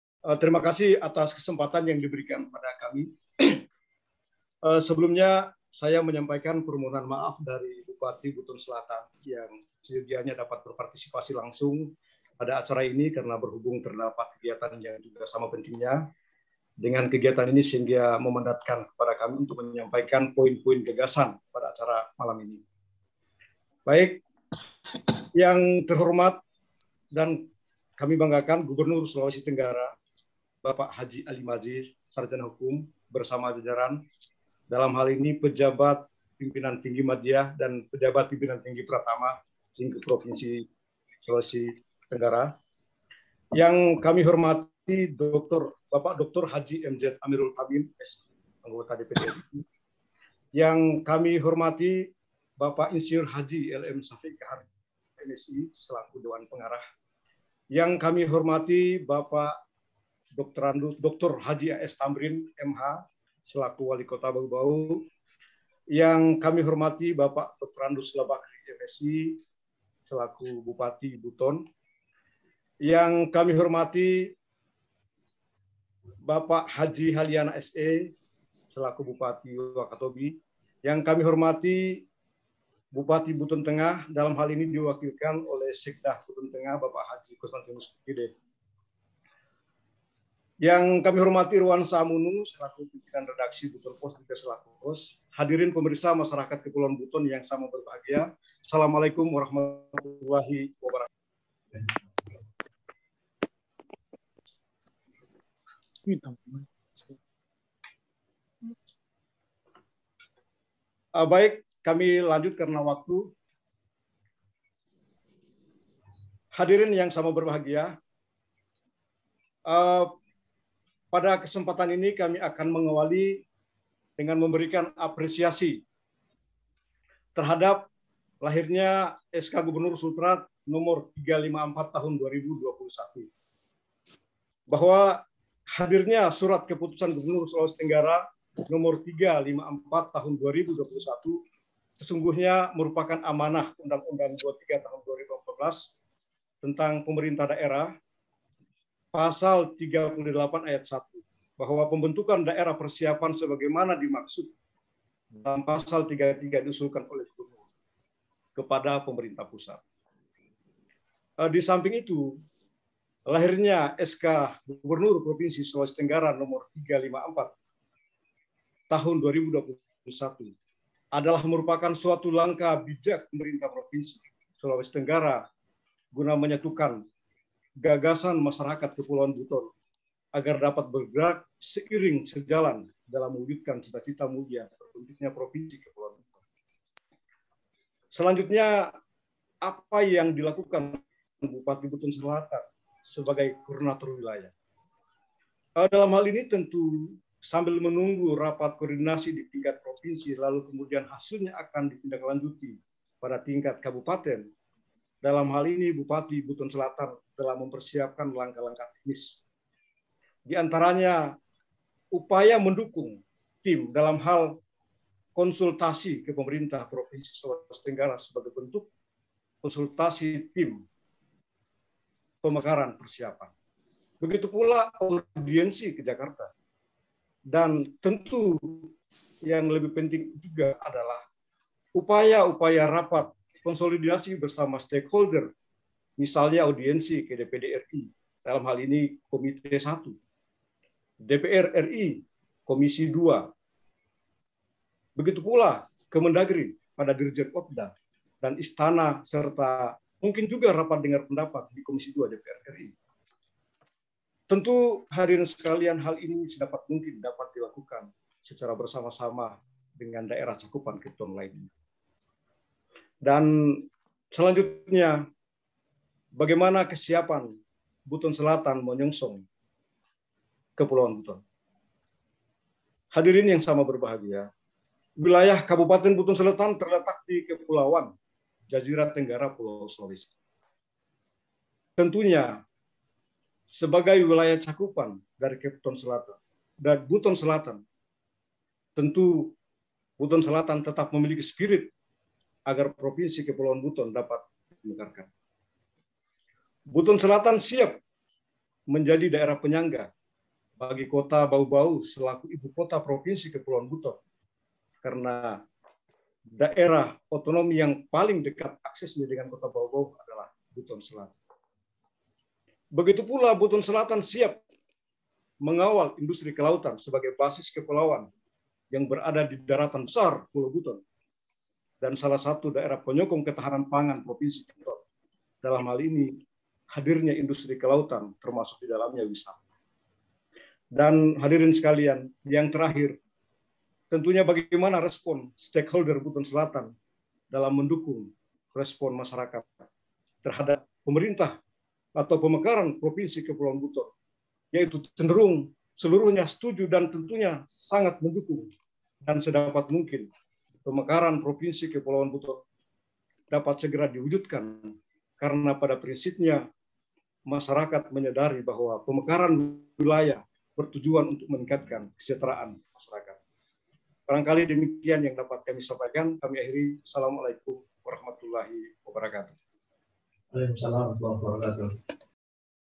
LAJU KEPTON | Busel Siap Menyangga Ibukota Kepton
LAJU KEPTON

Tema : "Perjuangan Kepton setelah SK Gubernur Sultra No 354/2021"

Membahas terkait upaya persiapan dan percepatan pembentukan provinsi KEPTON

LAJU
Adalah salah satu segmen dalam Channel YouTube CatatanIrwansyahAmunu yang membahas isyu-isyu update terhangat.